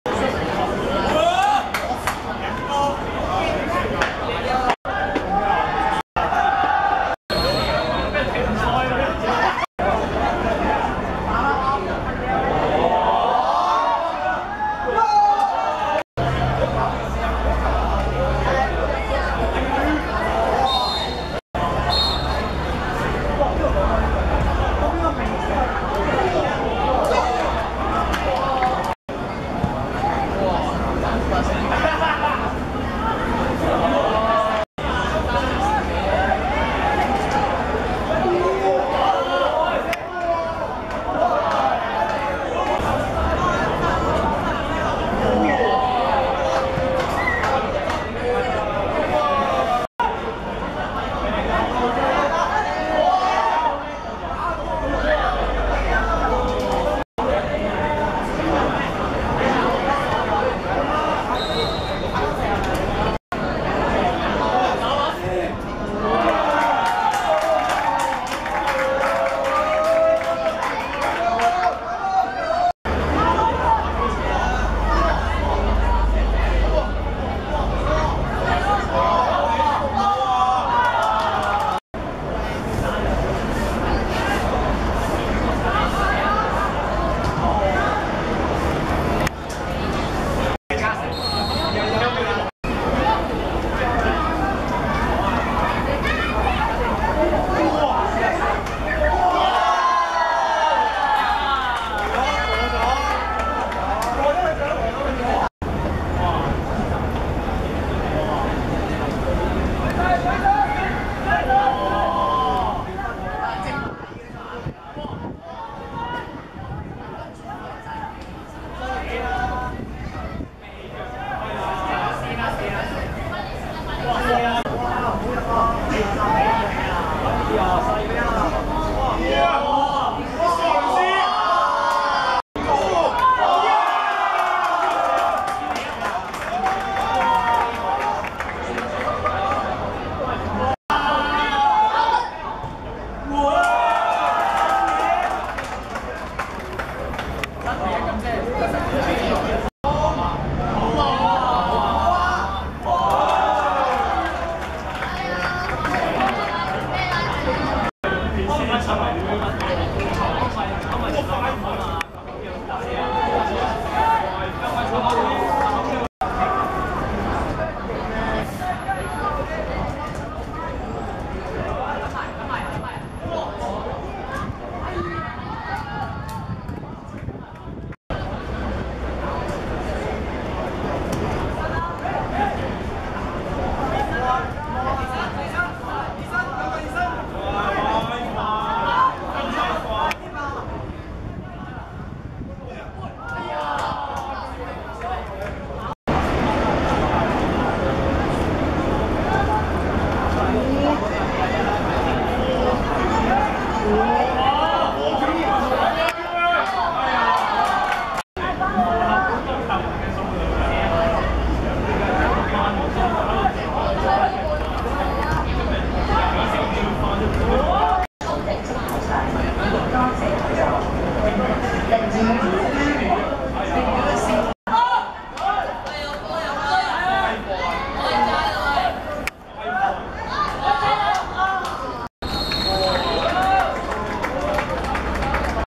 李琳明vs沈香林(2015.10.8.屯門學界足球甲組)精華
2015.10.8.兆麟運動塲 18:30 李琳明(綠) 1 : 4 沈香林(黃)